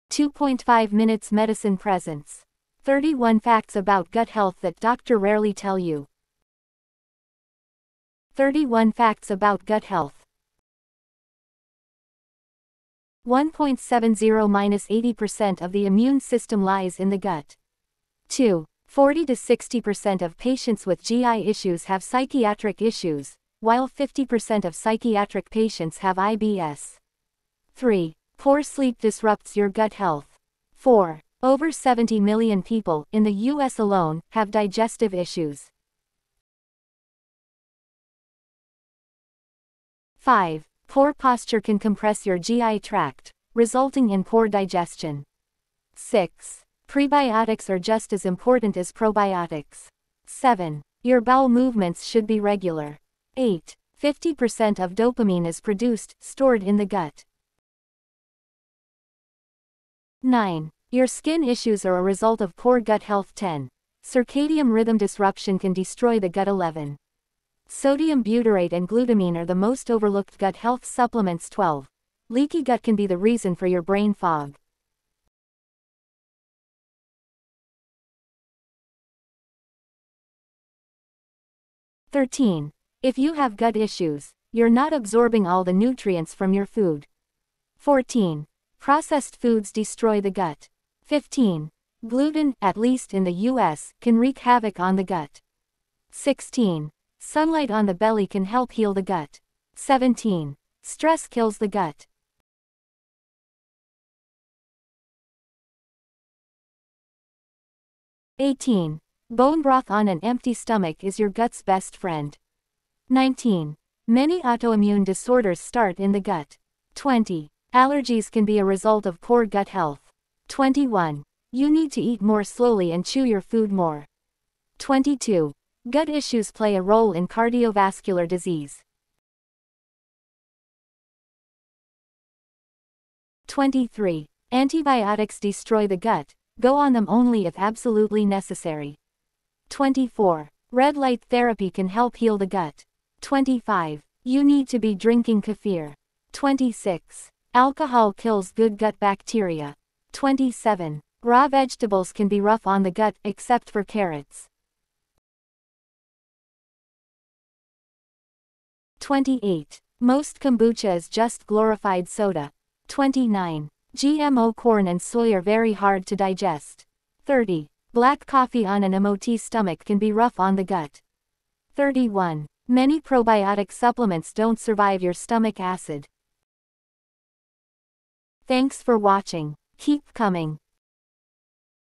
31 untold FACTS about Gut Health that DOCTOR rarely tell you | Act now before its late
31 facts about gut health that most doctors won’t tell you:
1.70-80% of the immune system lies in the gut
2. 40-60% of patients with GI issues have psychiatric issues, while 50% of psychiatric patients have IBS
3. Poor sleep disrupts your gut health
4. Over 70 million people (in the US alone) have digestive issues
5. Poor posture can compress your GI tract, resulting in poor digestion
6. Prebiotics are just as important as probiotics
7. Your bowel movements should be regular
8. 50% of dopamine is produced/stored in the gut
9. Your skin issues are a result of poor gut health
10.Circadium rhythm disruption can destroy the gut
11. Sodium butyrate and glutamine are the most overlooked gut health supplements
12.Leaky gut can be the reason for your brain fog
13.If you have gut issues, you’re not absorbing all the nutrients from your food
14.Processed foods destroy the gut
15.Gluten (at least in the US) can wreak havoc on the gut
16.Sunlight on the belly can help heal the gut
17.Stress kills the gut
18.Bone broth on an empty stomach is your guts best friend
19.Many autoimmune disorders start in the gut
20. Allergies can be a result of poor gut health
need to eat more slowly and chew your food more
22.Gut issues play a role in cardiovascular disease
23. Antibiotics destroy the gut, go on them only if absolutely necessary
24. Red light therapy can help heal the gut
25. You need to be drinking kefir
26. Alcohol kills good gut bacteria
27. Raw vegetables can be rough on the gut (except for carrots)
28. Most kombucha is just glorified soda
29. GMO corn and soy are very hard to digest
30. Black coffee on an emoty stomach can be rough on the gut
31. Many probiotic supplements don’t survive your stomach acid